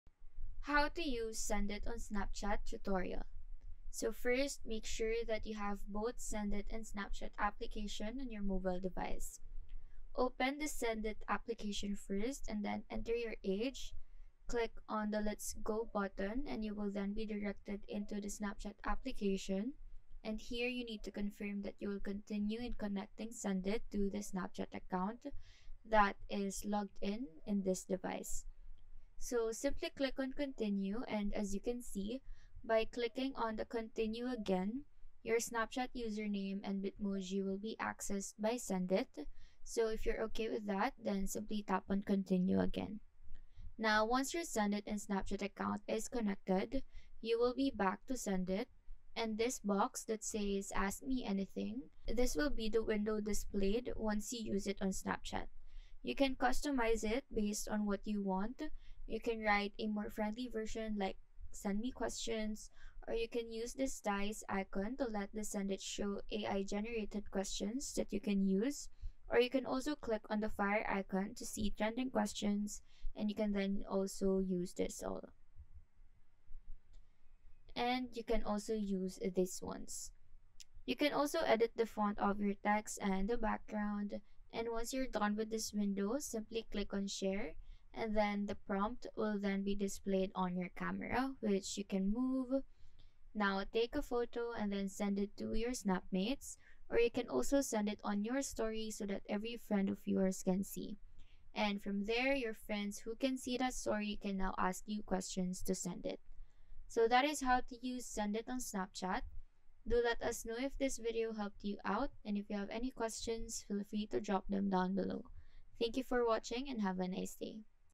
How To Use Sendit On Snapchat (Tutorial 2024)
How To Use Sendit On Snapchat

In this video, I will show you‎‎‏‏‎ ‎how to use sendit on snapchat